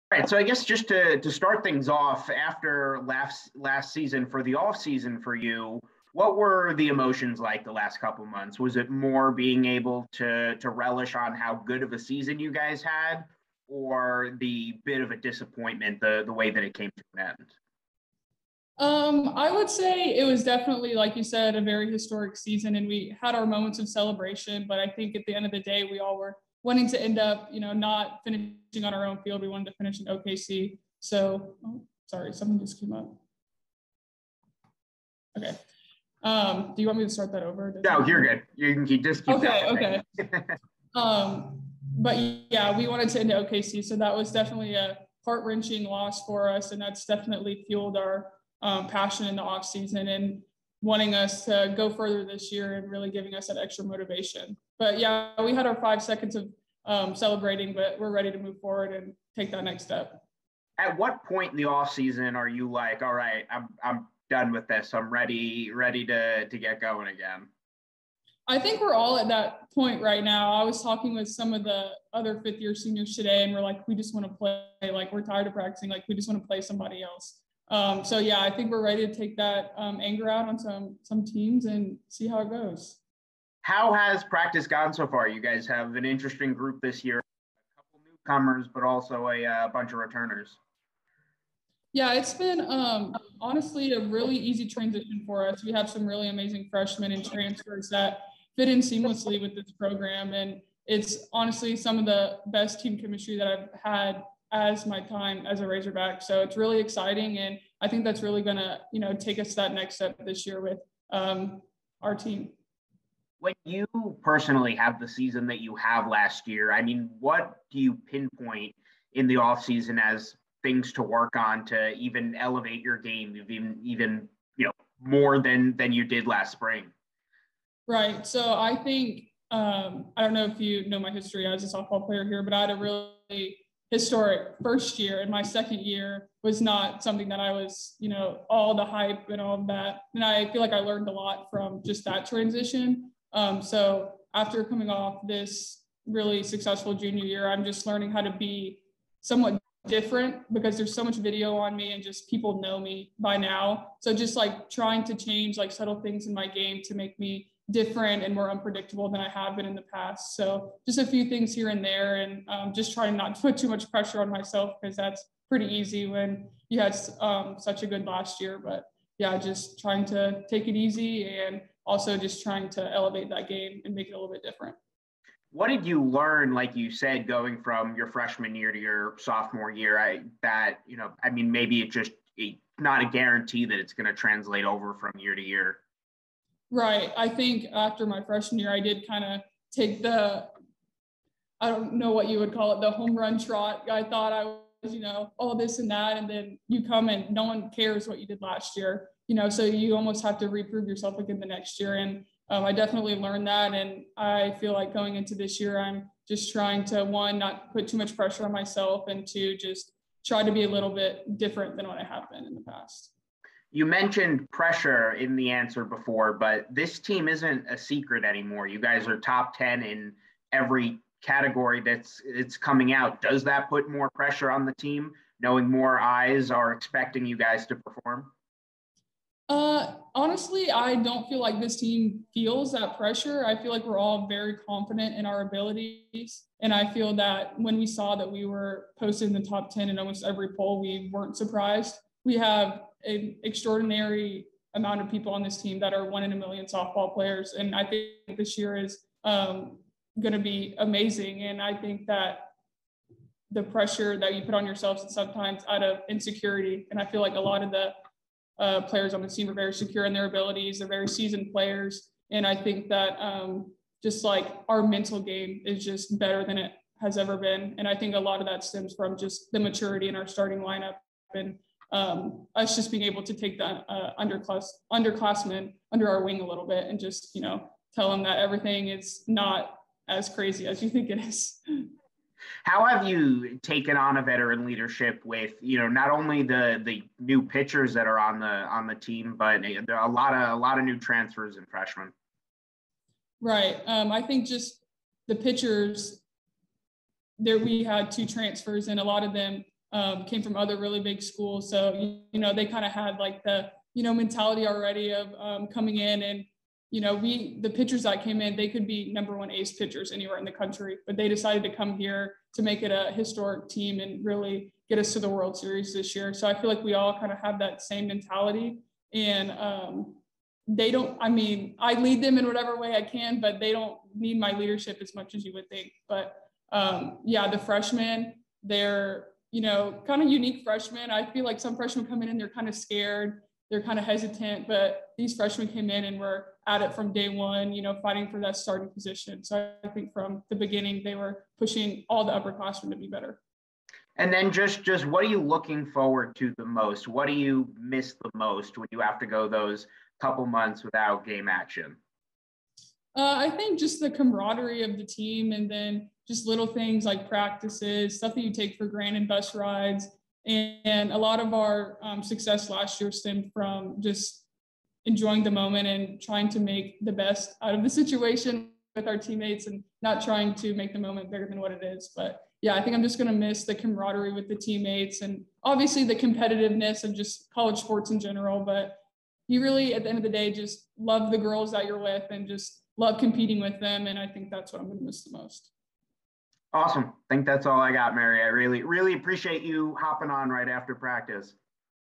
Previewing the Arkansas softball season with Mary Haff
The reigning SEC pitcher of the year previews the upcoming Arkansas softball season